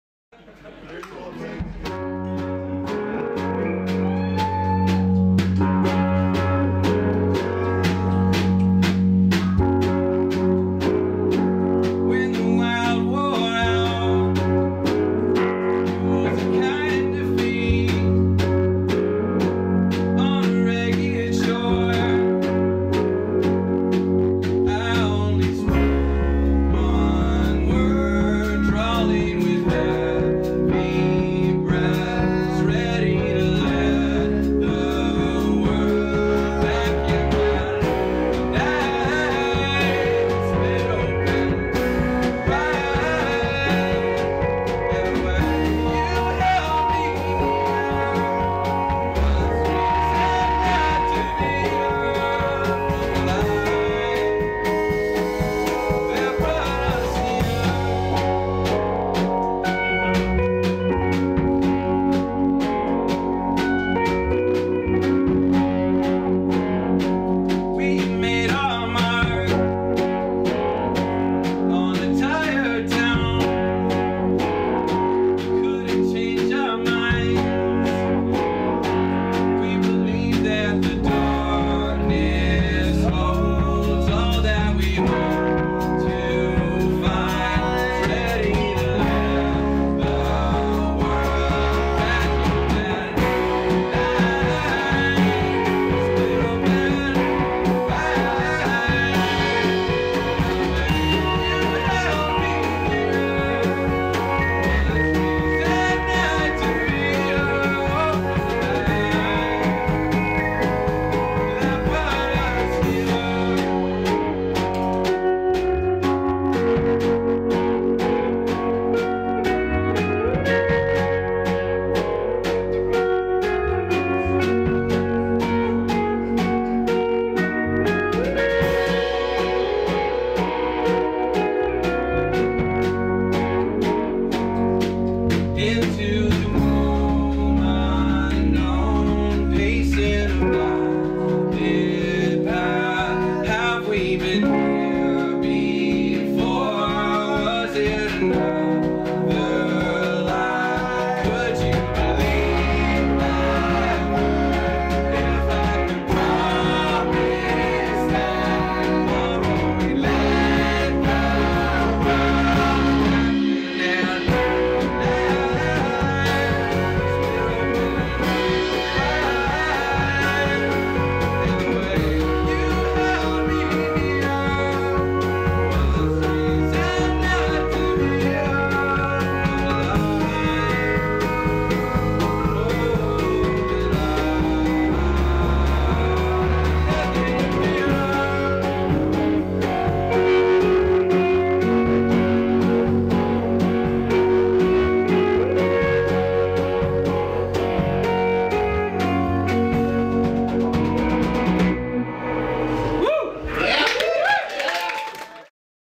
The Great Bloomers (Live in Miramichi)
On February 26th, 2012, The Wooden Sky played a house show in Miramichi, NB.  The Great Bloomers came along for the ride.